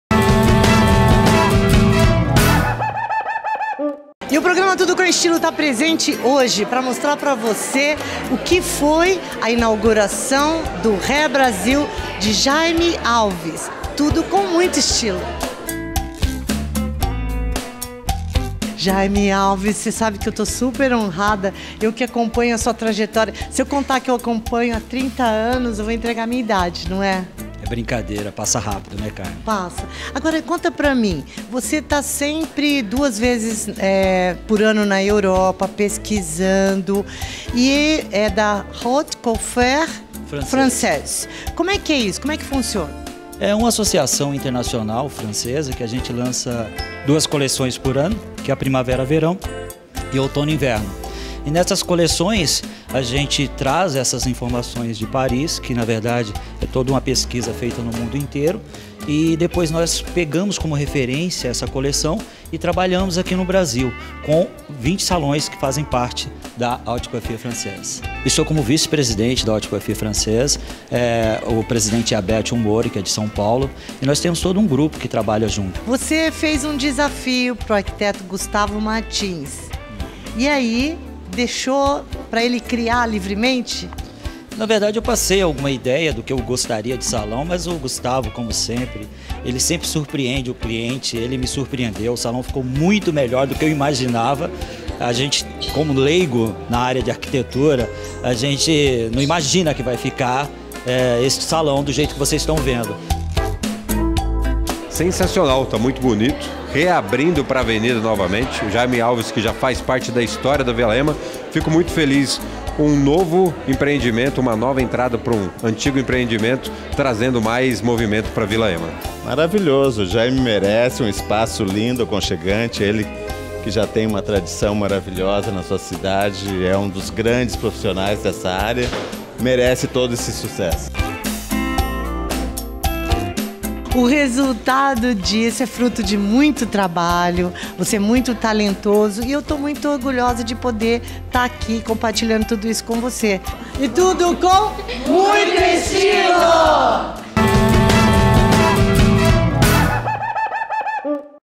JAIME ALVES - HAIR BRAZIL: INAUGURAÇÃO
SALÃO HAIR BRAZIL: Inauguração em Jan/18, em badalado evento com clientes, amigos e imprensa.Confira melhores momentos.